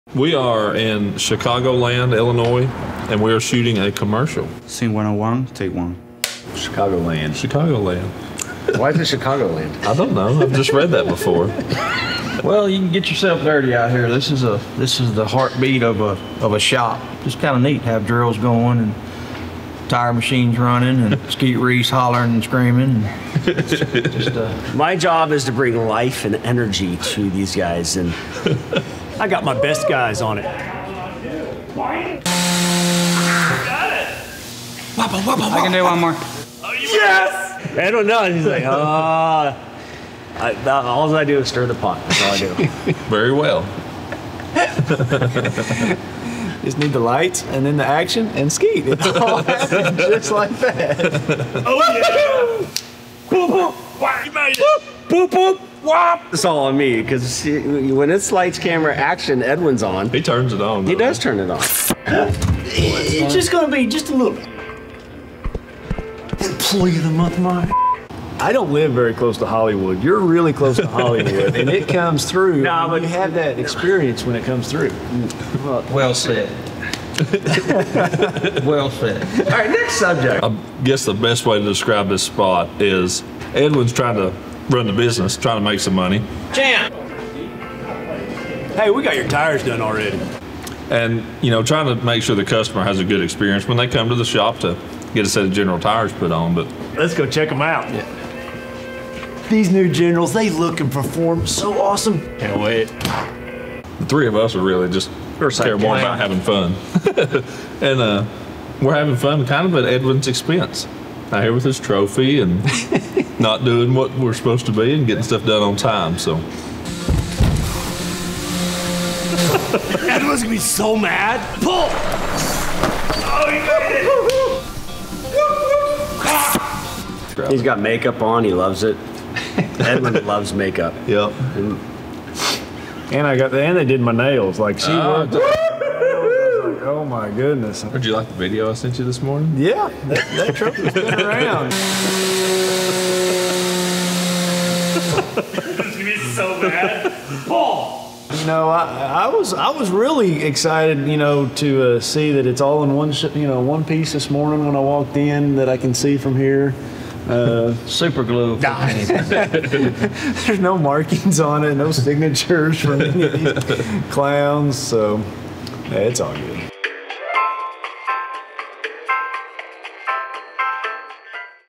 Behind The Scenes: The Team GT Fishing Commercial
The Team GT Fishing boys got together to film the latest commercial, but you can't get Edwin, Ott, Mark, and Skeet together and expect there not to be competition... and shenanigans! In this behind the scenes segment, they chat about the day of filming!